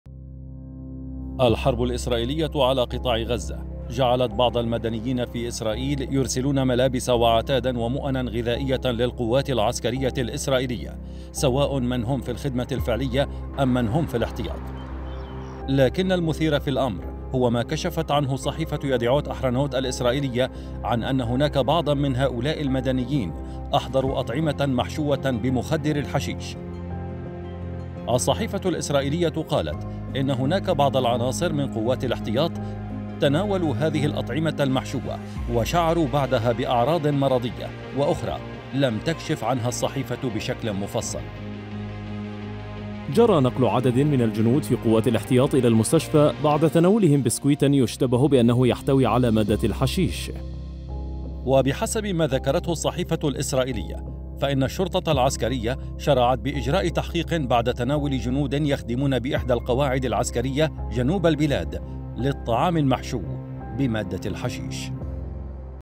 شعروا بعدها بأعراض مرضية.. جنود إسرائيليون يتناولون طعاما محشوا ب"مخدر الحشيش"
نقل عدد منهم إلى المستشفى.."يديعوت أحرونوت": جنود إسرائيليون تناولوا طعاما محشوا ب"مخدر الحشيش".. والشرطة العسكرية تفتح تحقيقا